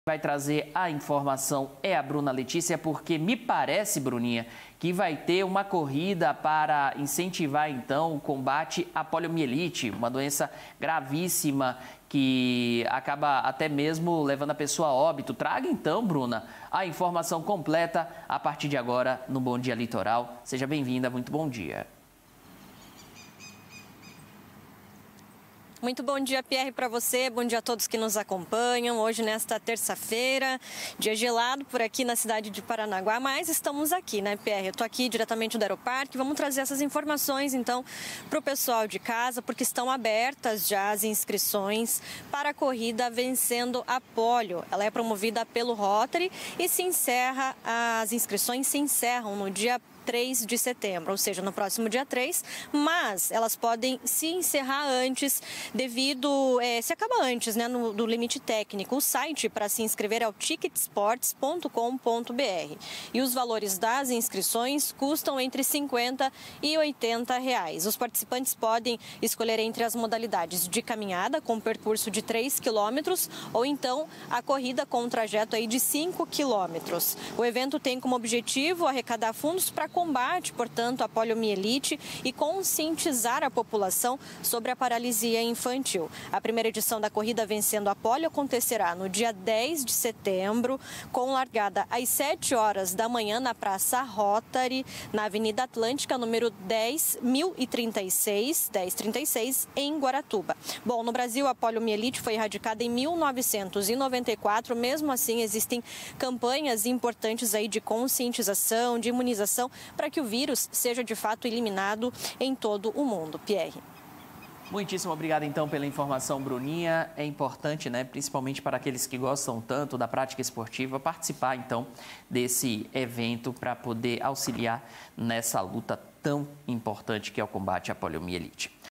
COMBATE À POLIOMIELITE
Rotary promove 1ª edição de corrida "vencendo a pólio" no próximo mês